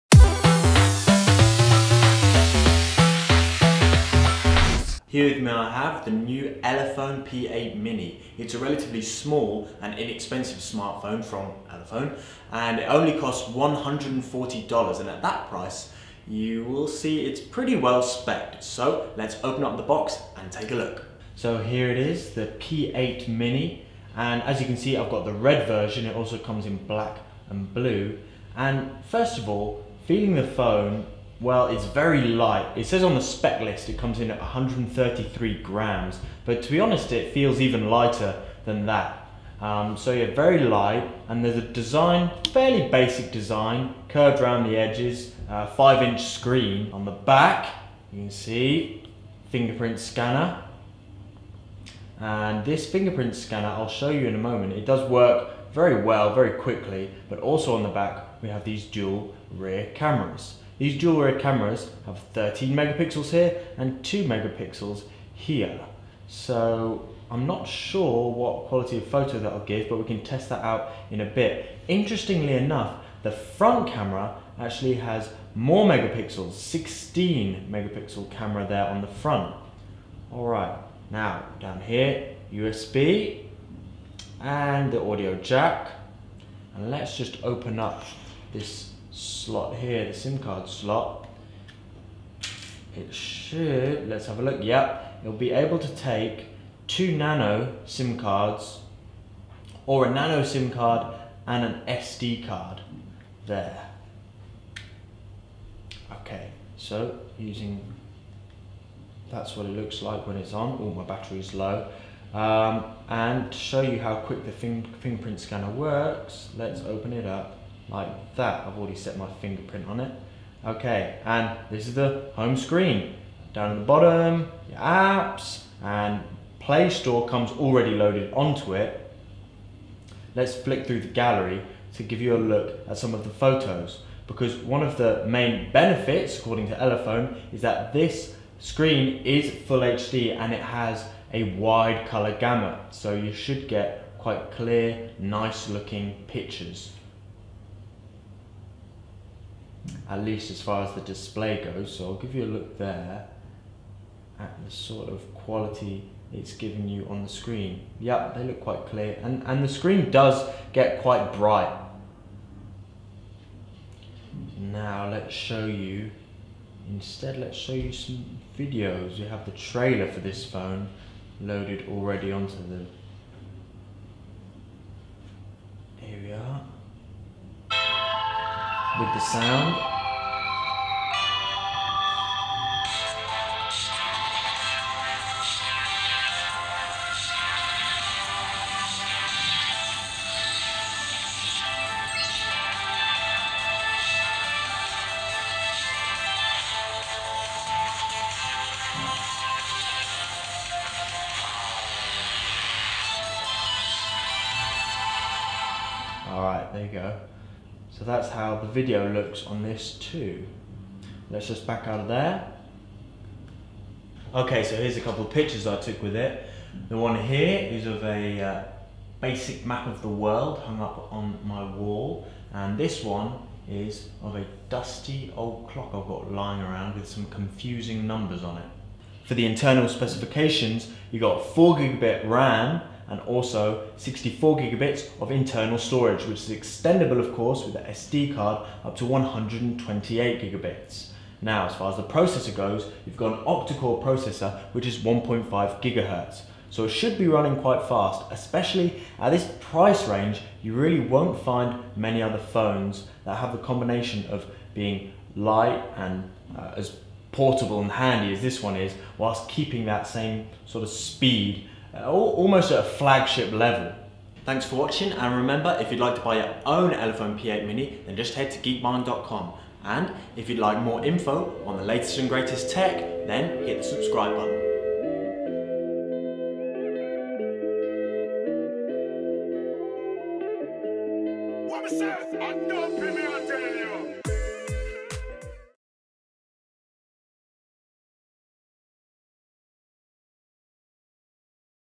Elephone P8 Mini Hands-On Review
Let's see what Elephone's new budget smartphone is like.
Correction: I said gigabits (Gb), and meant gigabytes (GB)